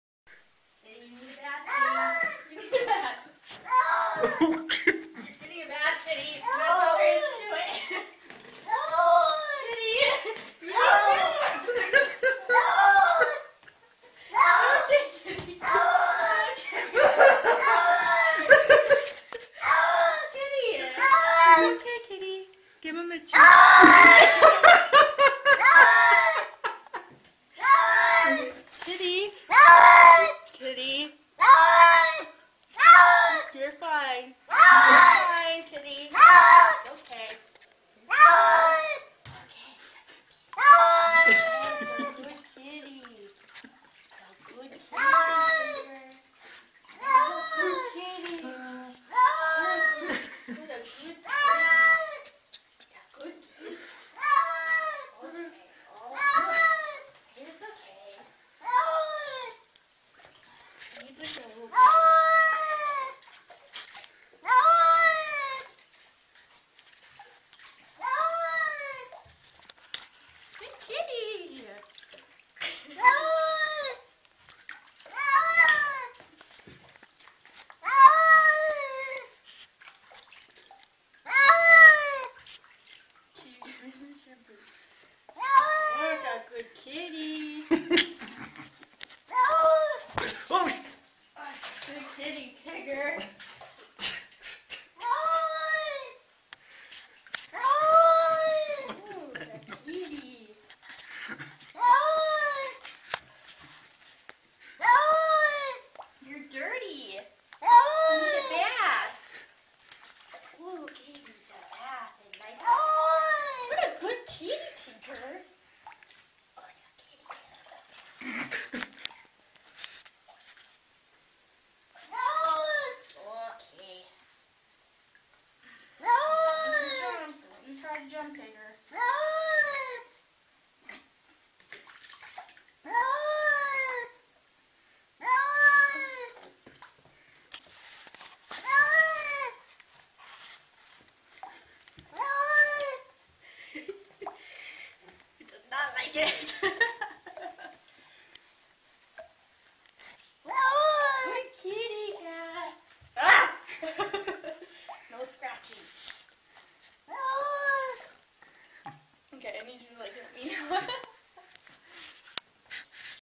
Suyu Görünce Davut Diye Bağıran Kedi
Suyu sevmeyen kedinin serzenişi güldürüyor.